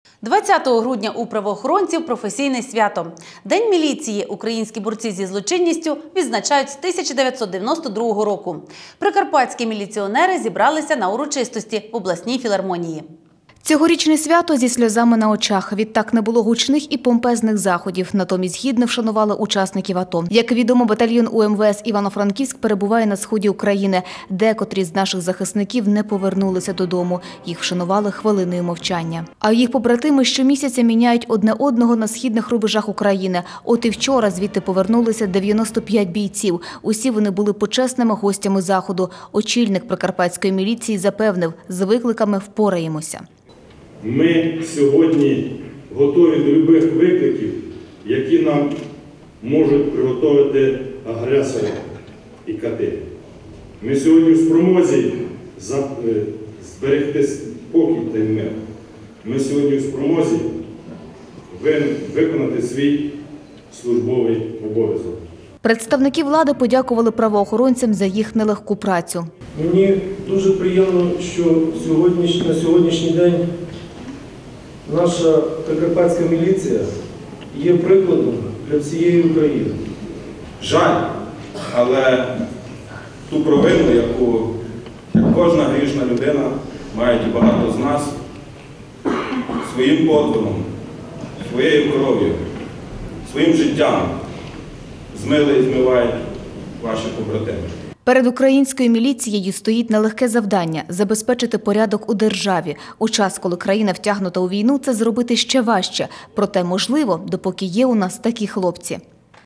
2014 12 19 День міліції
20 грудня у правоохоронців професійне свято. День міліції українські борці зі злочинністю відзначають з 1992 року. Прикарпатські міліціонери зібралися на урочистості в обласній філармонії.